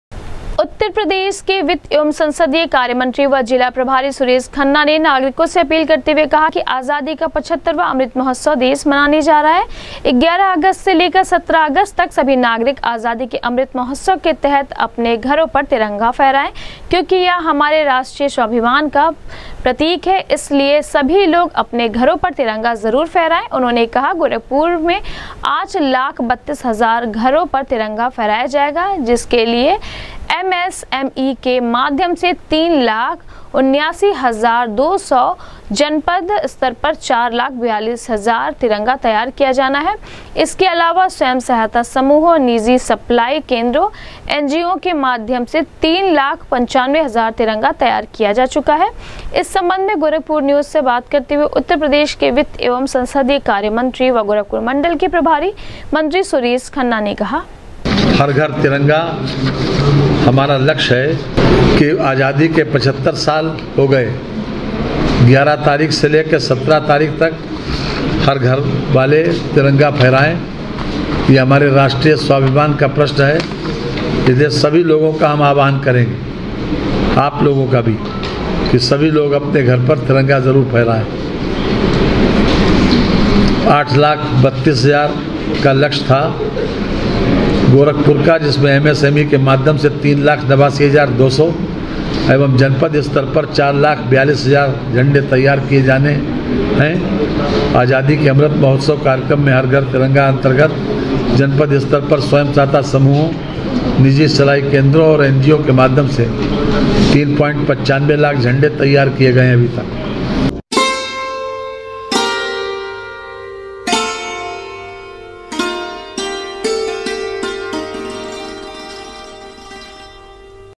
ग्यारह अगस्त से सत्रह अगस्त तक सभी नागरिक अपने घरों पर हर घर तिरंगा कार्यक्रम के तहत फहराये तिरंगा
ग्यारह अगस्त से सत्रह अगस्त तक सभी नागरिक अपने घरों पर हर घर तिरंगा कार्यक्रम के तहत फहराये तिरंगा, प्रदेश के वित्त एवं संसदीय कार्य मंत्री व गोरखपुर मण्डल के प्रभारी मंत्री सुरेश खन्ना ने नागरिको से की अपील